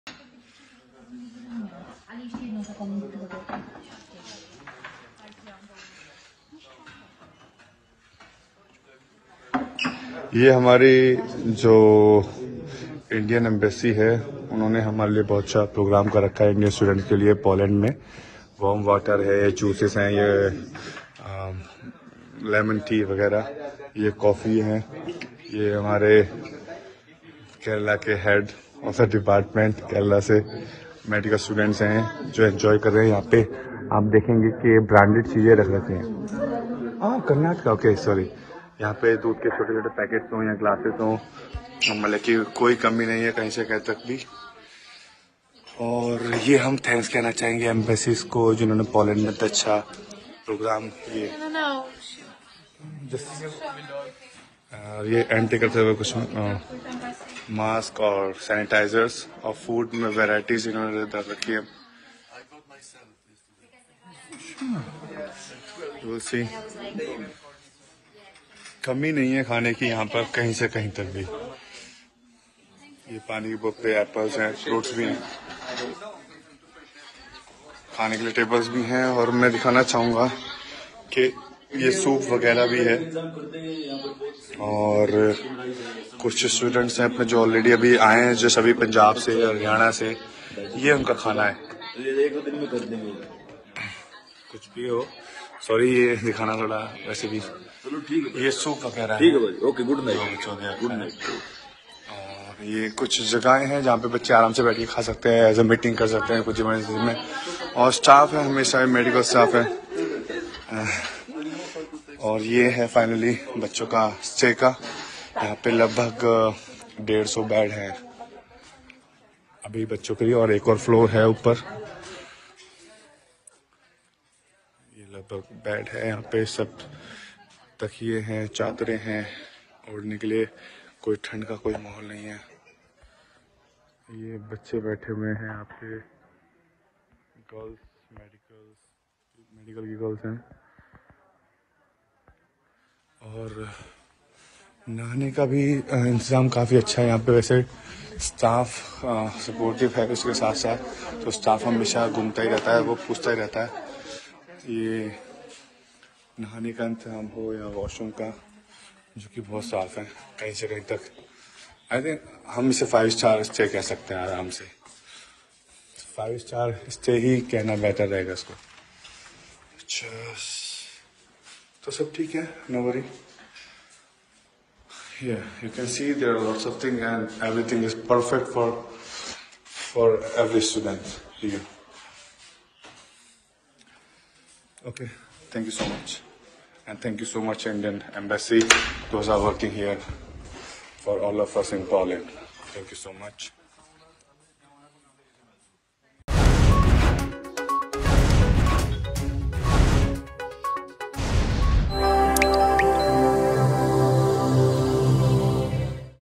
Russia Ukraine War: Food and Accommodation for Indian Students crossing into Poland from Ukraine
Food and Accommodation for Indian Students who were taking shelter in Poland amid Russia Ukraine War. Watch this report to know what they will eat and where they will sleep.